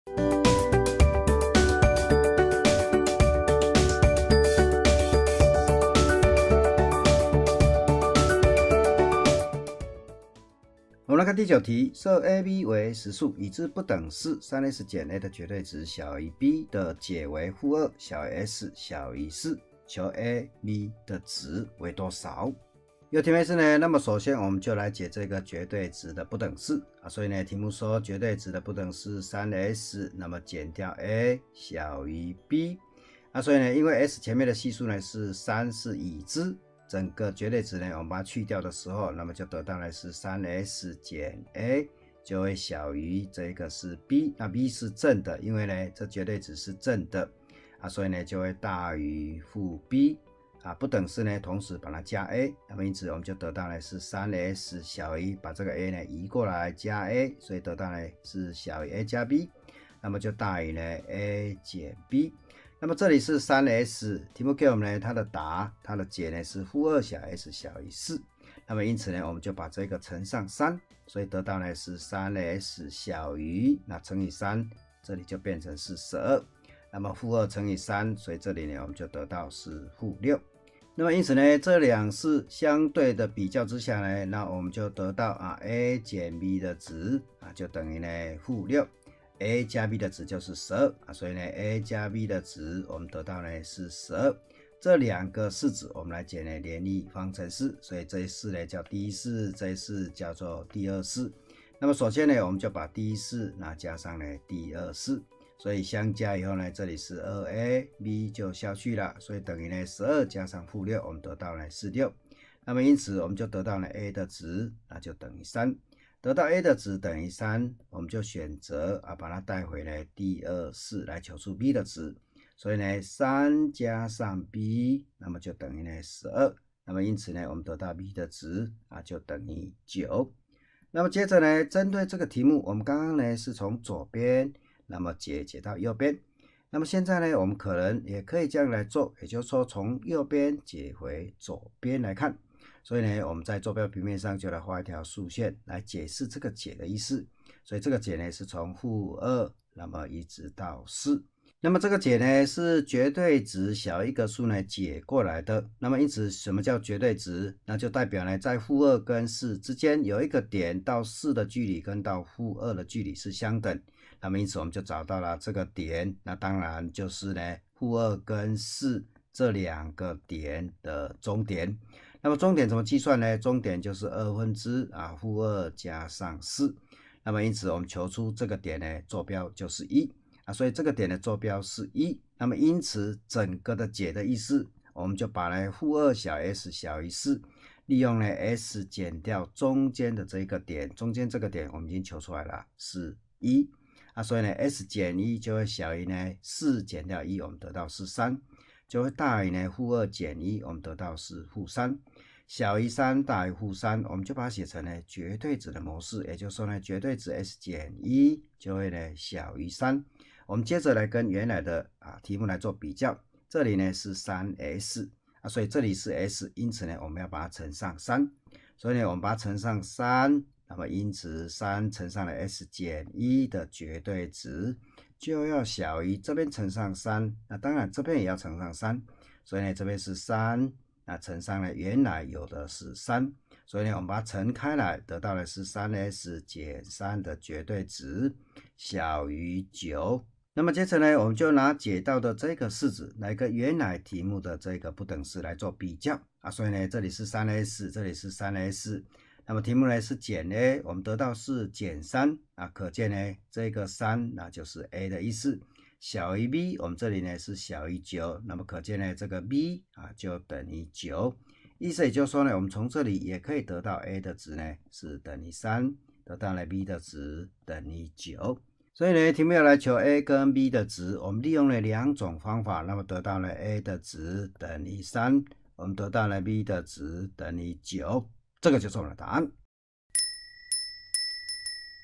高中數學第一冊 1-1習題 第9題
由陳建在老師精心說明，請參考高中數學第一冊課本，高中數學第一冊 1-1習題 第9題。